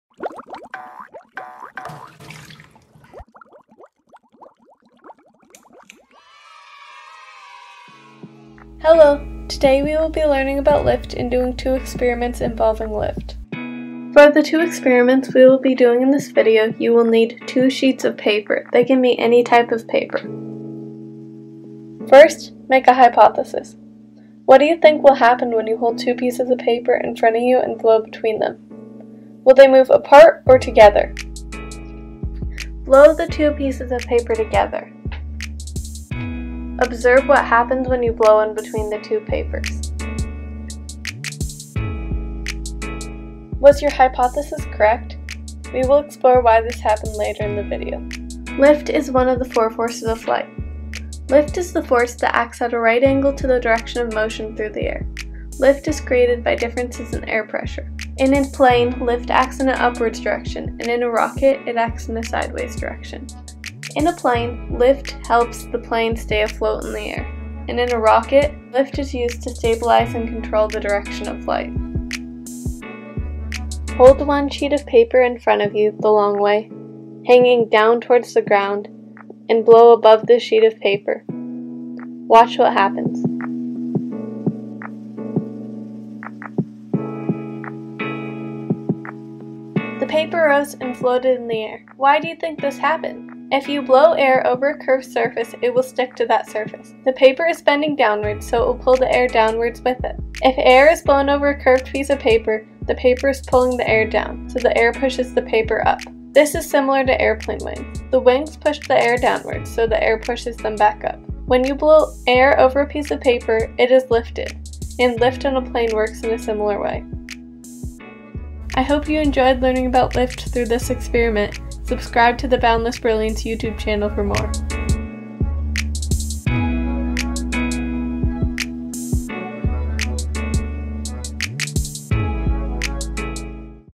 What is Lift? | Easy Science Experiment
Are you an aspiring astronaut or aeronautical engineer? Do you just want to learn more about the science and physics behind rockets? Want to have some fun? If so, you are in the perfect place!

Follow this experiment to learn about the concept of lift!

FAQ:
Where does BB have chapters? Occidental College, Azusa-Pacific University, and Arizona State University